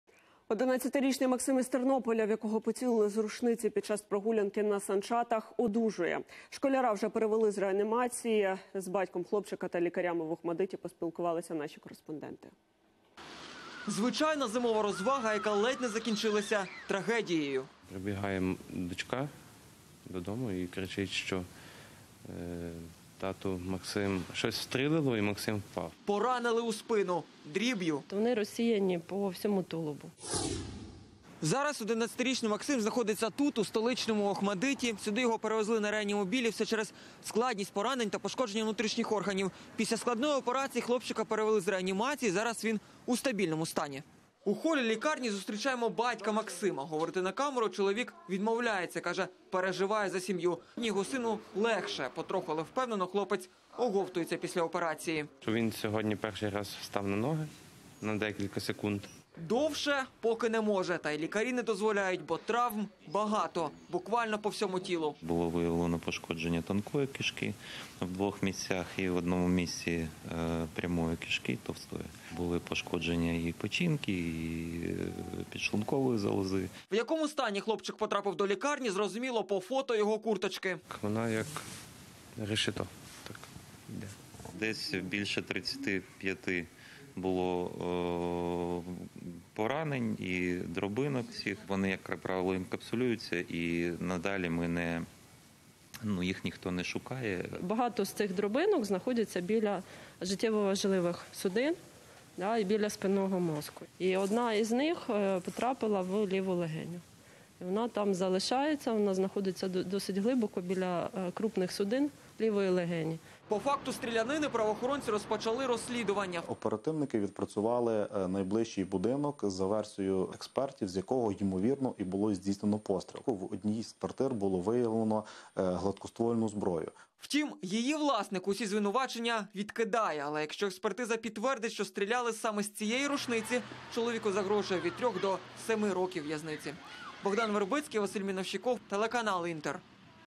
Стрілянина у Тернополі: постраждалого підлітка прооперували у лікарні "Охматдит"
11-річний Максим з Тернополя, в якого поцілили з рушниці під час прогулянки на санчатах, - одужує